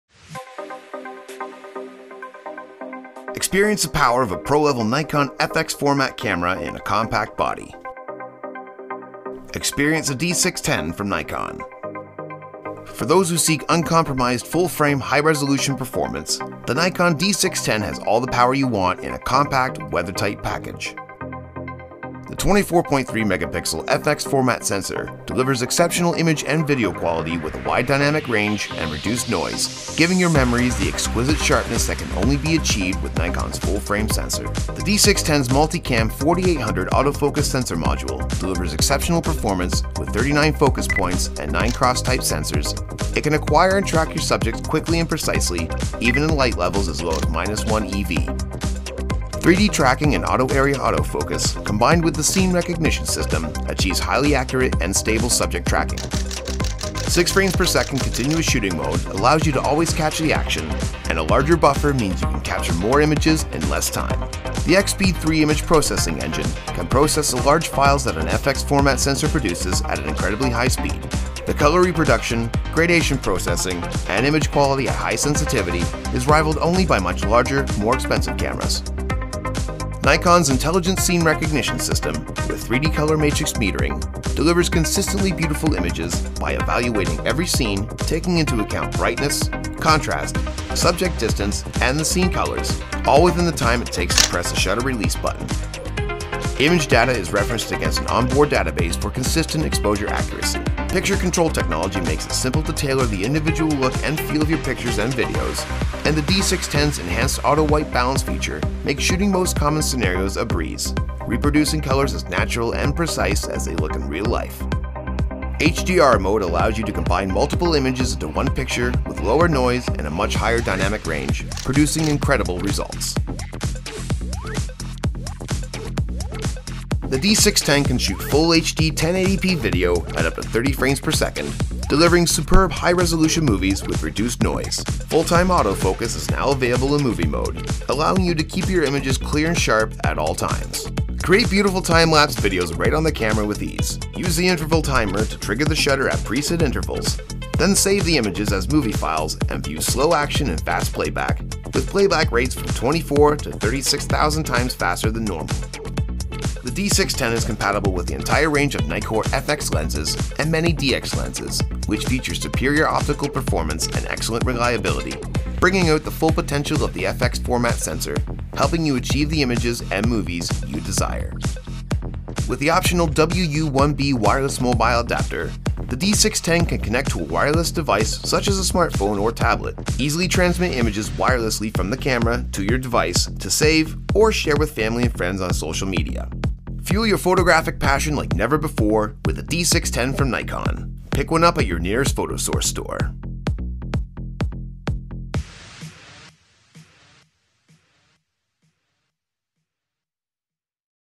Nikon D610
Experience the passion of Nikon full-frame photography.

The power of a pro-level Nikon FX-format camera—dazzling full-frame images, broadcast-quality 1080p video at your fingertips, superior low-light performance, faster frame rate up to 6 frames-per-second (fps), tack sharp autofocus with 39 focus points, built-in flash with Commander Mode, wireless photo sharing and much more—is attainable in a compact, lightweight full-frame HD-SLR body. A new continuous quiet mode lets you capture a sequence of shots without the noise associated with the raising and lowering of the D-SLR's mirror. Optimized for full-frame shooting, streamlined for compactness and value, the D610 will fuel your photographic passion like never before.